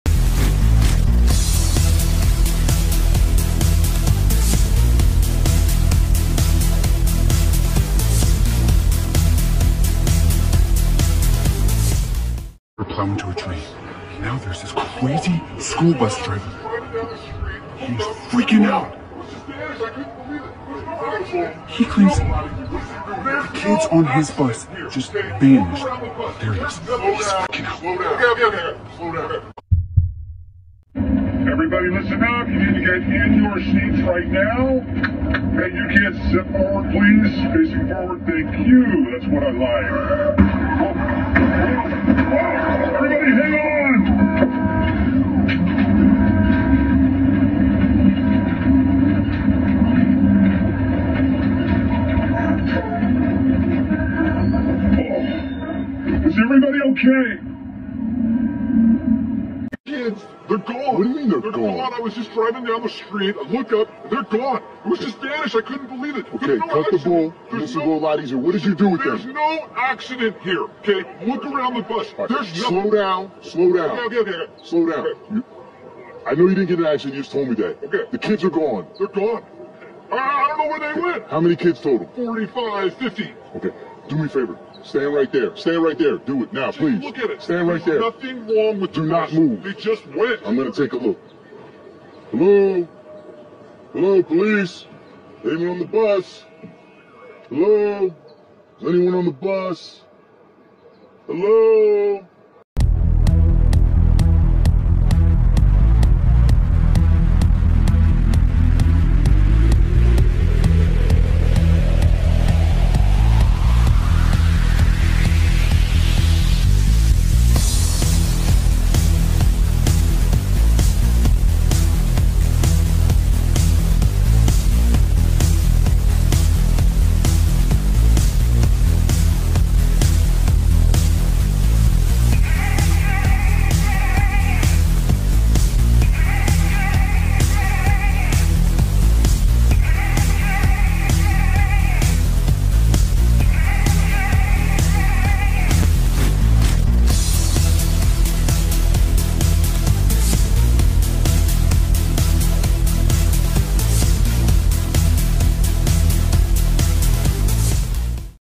💥El BUS DE LA MATRIX
CONTEXTO: Recuerdan el vídeo de los niños de la Matrix? ese  incidente donde muchos niños abordo de un camión escolar desaparecen en forma instantánea y que aún no pueden encontrar?
NOW Les traemos este nuevo REVIEW con nuevos ángulos del incidente y la conmoción que ha causado en las fallas de la Matrix